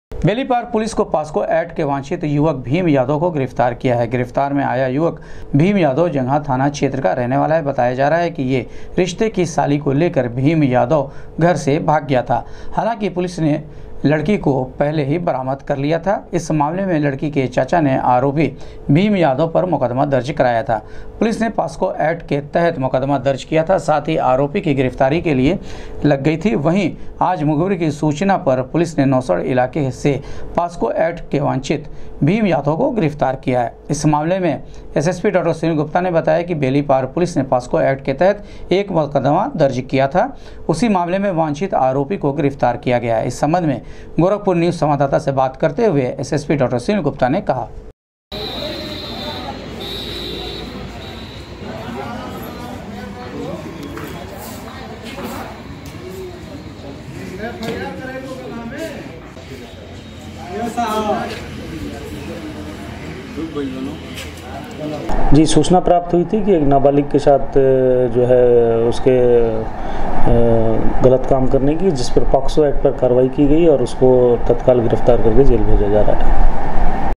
पास्को एक्ट के वांछित अभियुक्त को बेलीपार पुलिस ने किया गिरफ्तार, एसएसपी डॉ सुनील गुप्ता
पास्को एक्ट के वांछित अभियुक्त को बेलीपार पुलिस ने किया गिरफ्तार, एसएसपी डॉ सुनील गुप्ता ने कहा, आरोपी के विरुद्ध की जा रही है विधिक कार्यवाही।